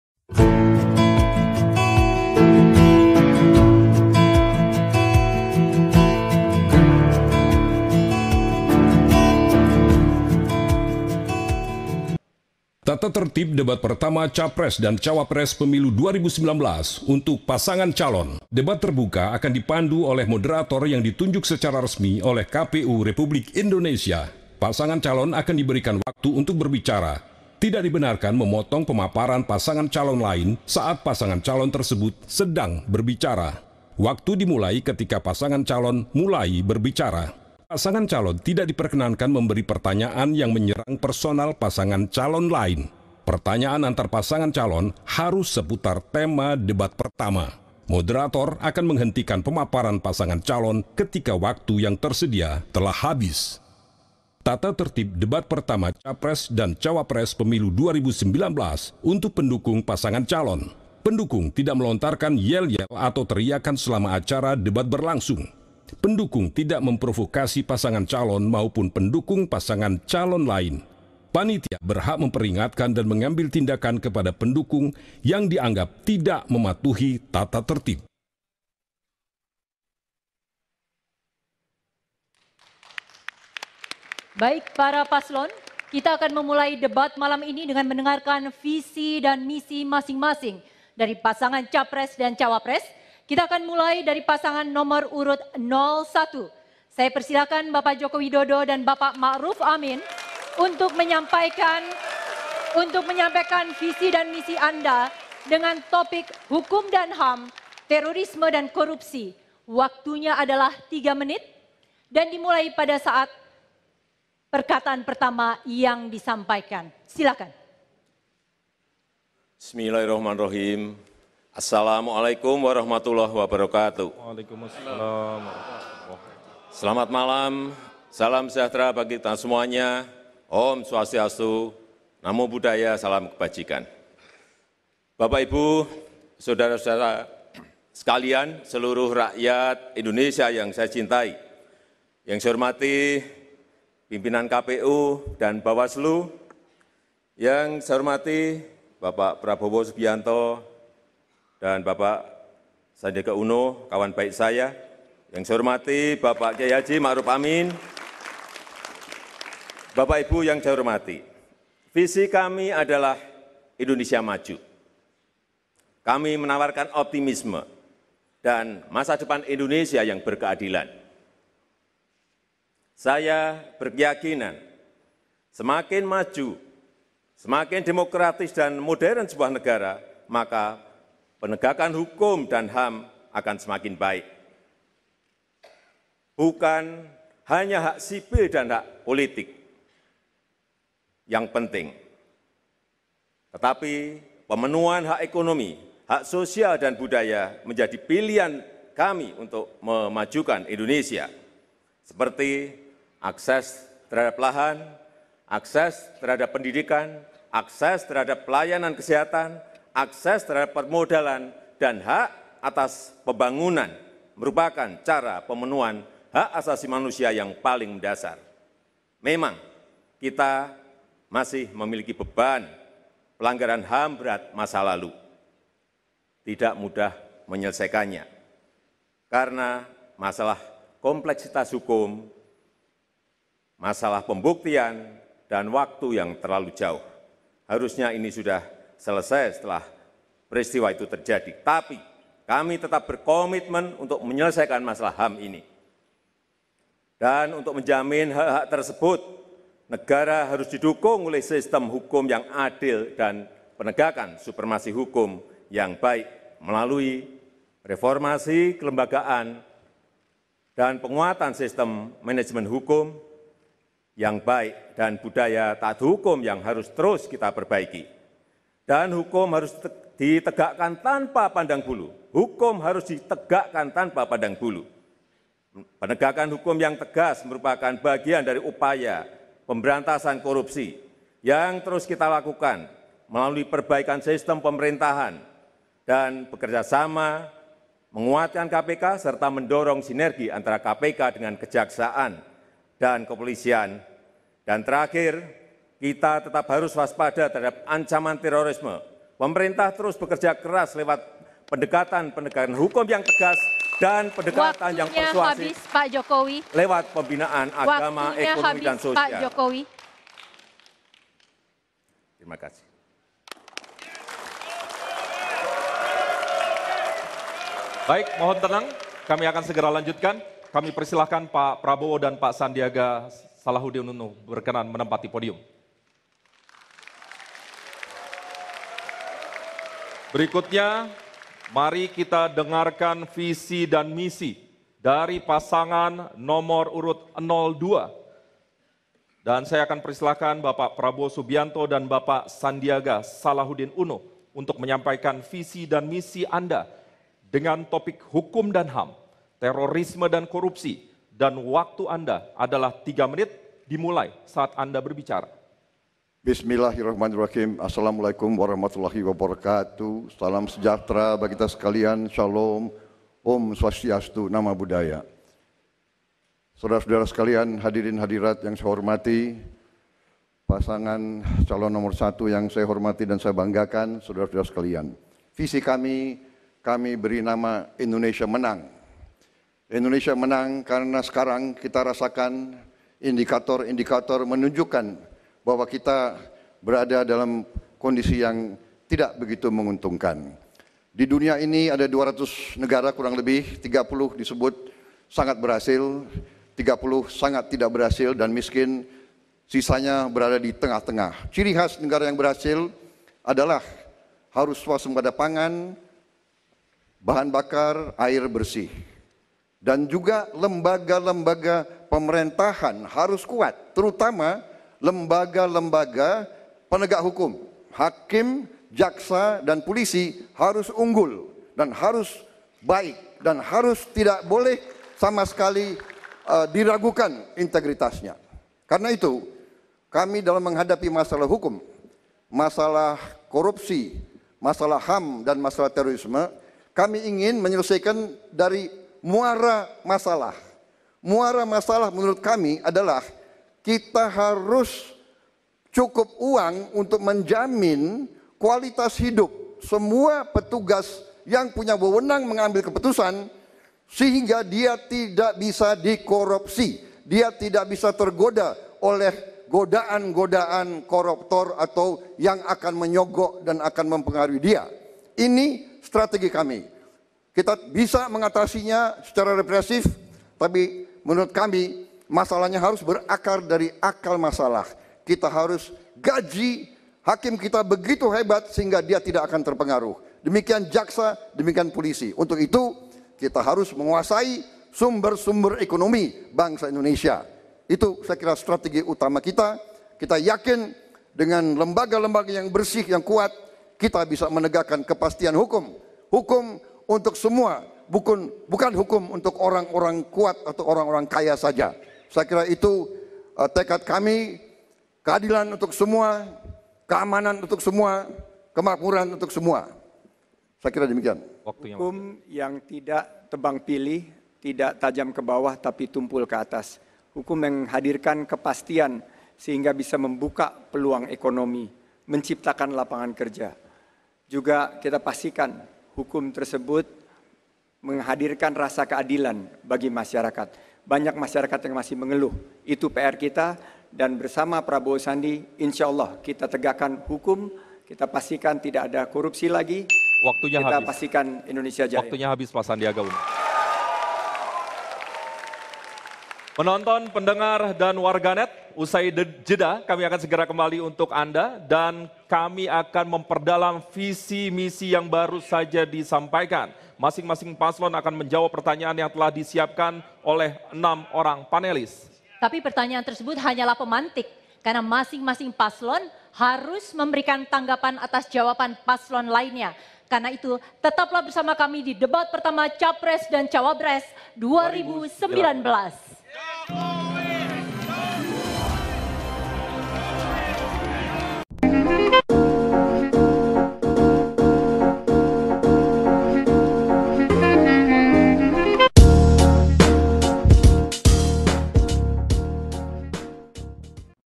Debat Pilpres: 'Datar,' diakhiri tanpa apresiasi sesama pasangan calon - MEMANAS
Debat perdana Pilpres 2019 berakhir dengan kedua pasangan calon menolak untuk mengapresiasi satu sama lain, bahkan sesudah moderator Ira Koesno dan Imam Priyono menyarankannya berkali-kali di babak pernyataan penutup.

Beberapa kali muncul situasi panas dan saling serang. Misalnya di tema korupsi, saat Prabowo tampak gelagapan menjawab pertanyaan Jokowi tentang partai yang dipimpinnya, Gerindra yang dalam catatan ICW, mengajukan enam mantan napi koruptor sebagai caleg.